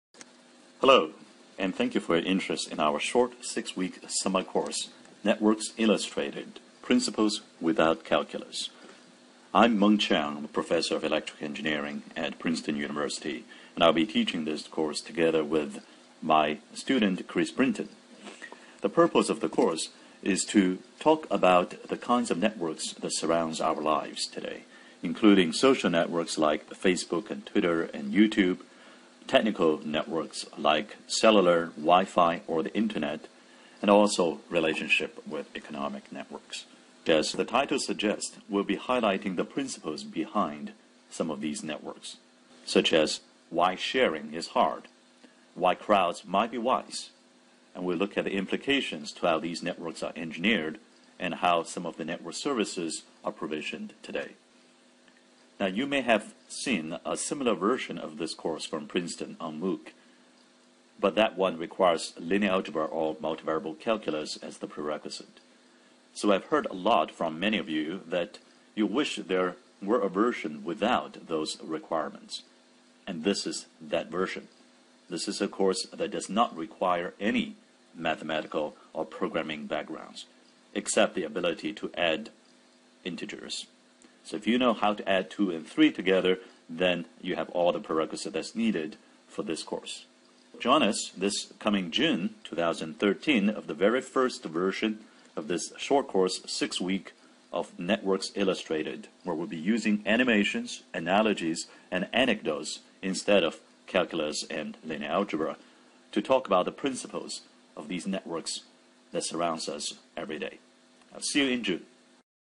Networks Illustrated: Principles without Calculus
This course serves as an introduction to the basic principles that govern all aspects of our networked lives. We will learn about companies like Google and technologies like the Internet in a way that requires no mathematics beyond basic algebra.